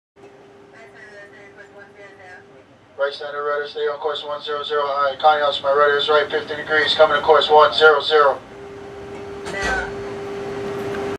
USS Kidd Sailors Keep Watch During MH370 Search
GULF OF THAILAND (March 11, 2014) Sailors assigned to the guided-missile Destroyer USS Kidd (DDG 100), keep watch as part of the search and rescue effort for the missing Malaysian airlines flight MH370. The flight had 227 passengers from 14 nations, mainly China, and 12 crew members. According to the Malaysia Airlines website, three Americans, including one infant, were also aboard. (U.S. Navy video/Released)